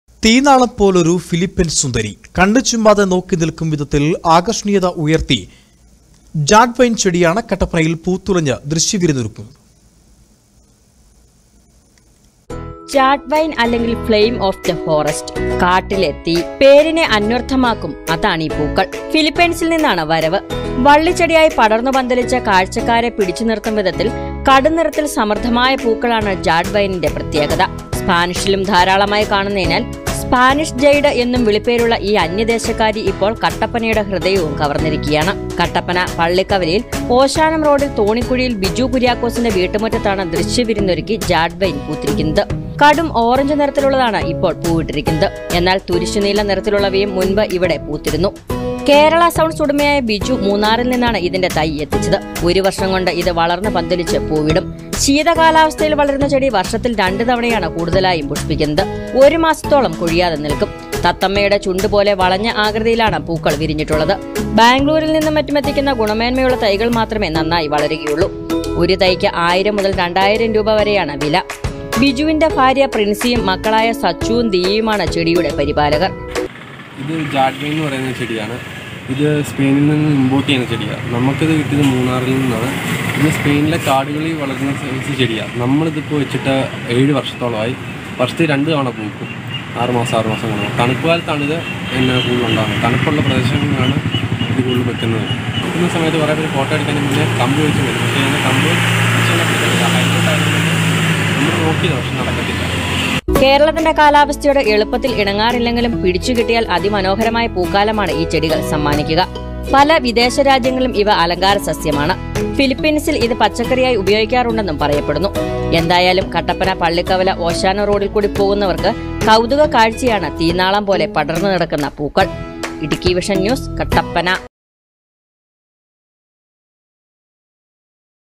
തീനാളം പോലൊരു ഫിലിപ്പൈന്‍ സുന്ദരി, പൂത്തുലഞ്ഞ് ജാഡ് വൈന്‍ ചെടി
തീനാളം പോലൊരു ഫിലിപ്പൈന്‍ സുന്ദരി, ജാഡ് വൈന്‍ ചെടിയാണ് കട്ടപ്പനയില്‍ പൂത്തുലഞ്ഞ് ദൃശ്യവിരുന്നൊരുക്കുന്നത്